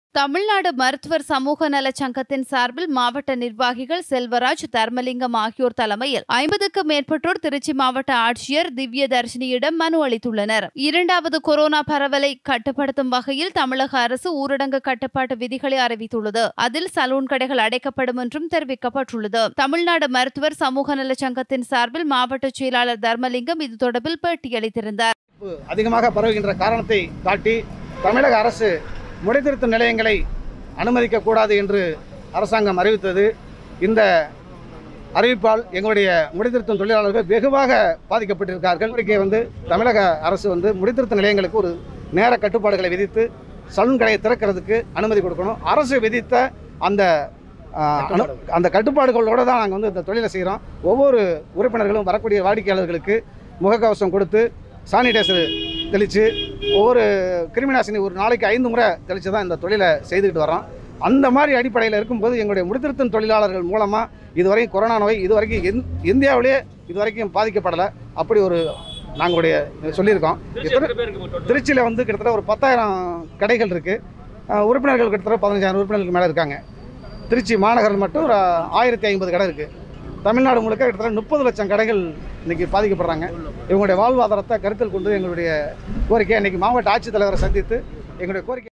திருச்சி மாவட்டத்தில் தொழிலாளர்களுக்கு வாழ்வாதரம் வழங்க வேண்டுமென கோரிக்கை.
திருச்சி மாவட்டத்தில் தொழிலாளர்களுக்கு வாழ்வாதரம் வழங்க வேண்டுமென தமிழ்நாடு மருத்துவர் சமூக நலச் சங்கத்தினர் கோரிக்கை.